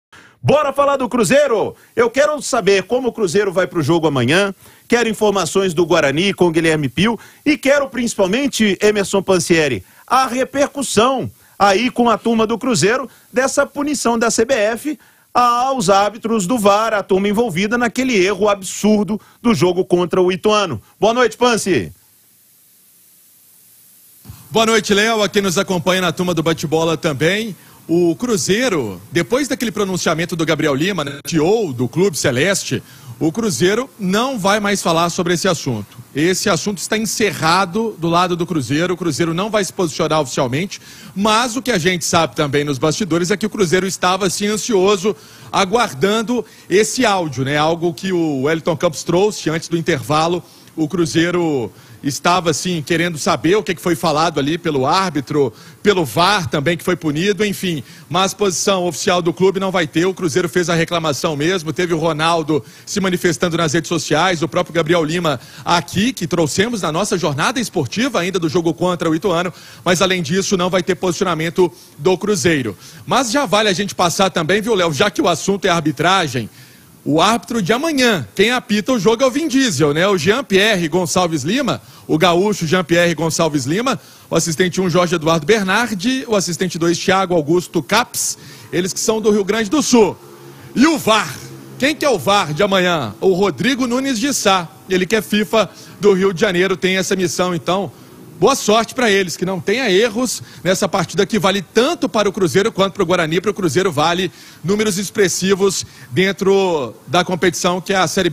CRUZEIRO NÃO IRÁ MAIS SE PRONUNCIAR SOBRE ERRO DO VAR
O repórter Emerson Pancieri traz novidades sobre as medidas tomadas pela diretoria do Cruzeiro em relação ao erro do VAR.